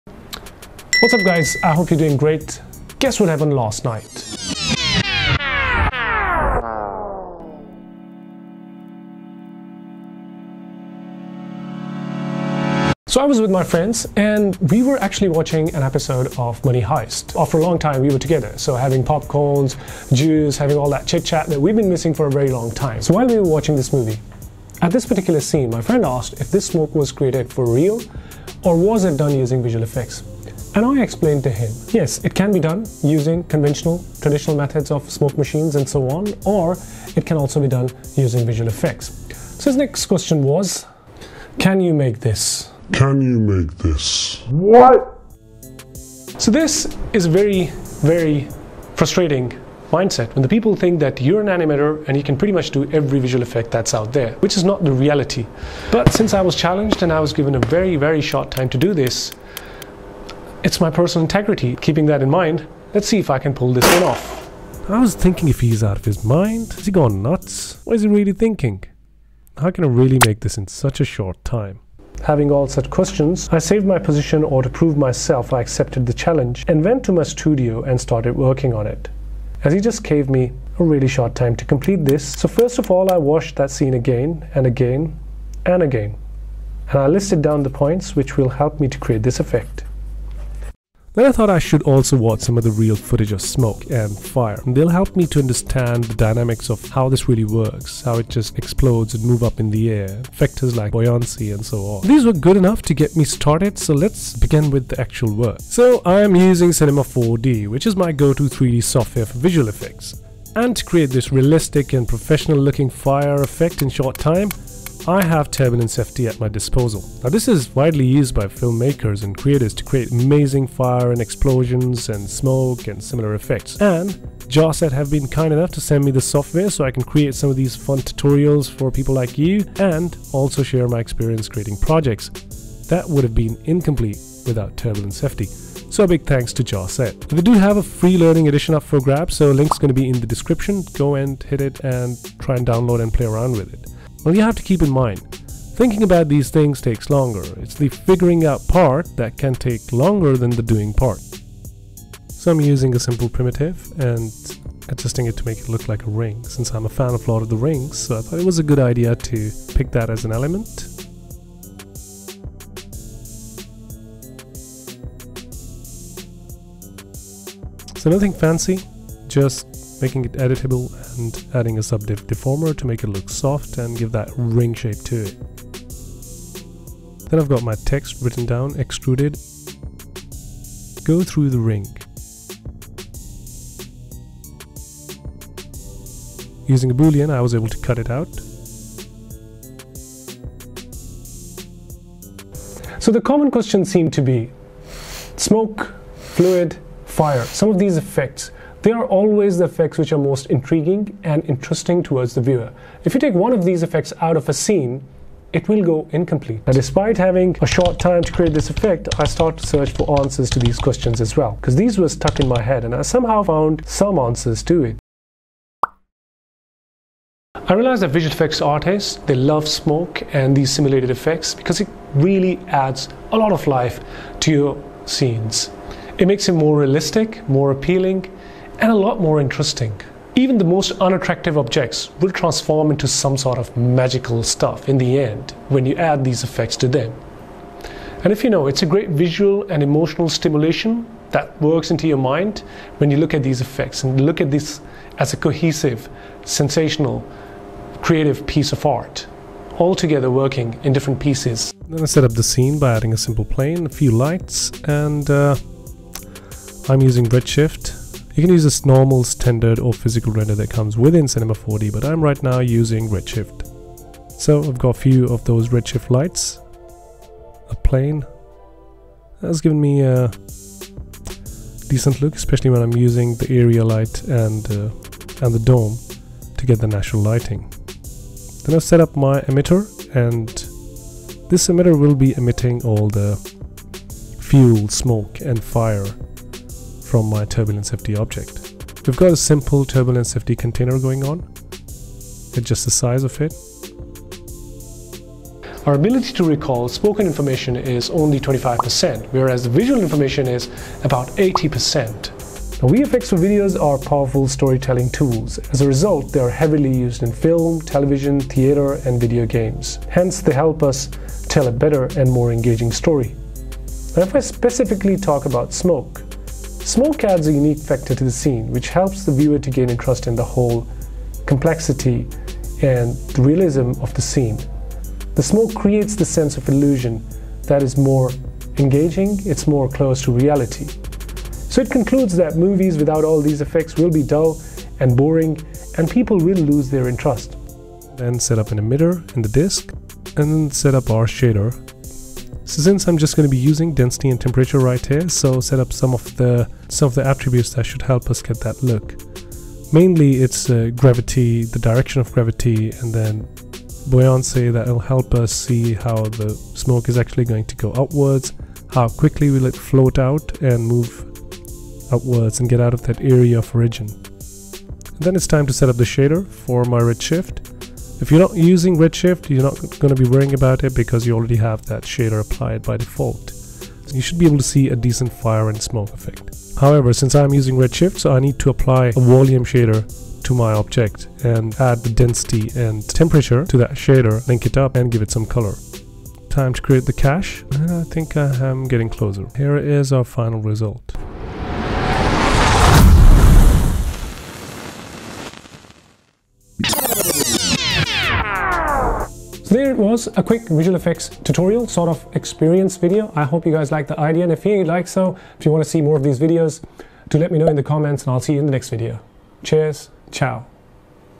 Making Realistic Fire Tutorial in 5 Hours: Turbulence FD and Cinema 4D
I was challenged by a friend to make realistic smoke and fire in just 5 hours.
Here it is a quick tutorial of making professional and realistic looking fire effect in Cinema 4d and it would be incomplete without Turbulence FD.
I have used Jawset Turbulence FD with Cinema 4D to create effects like smoke and fire.
So a big thanks to Jawset.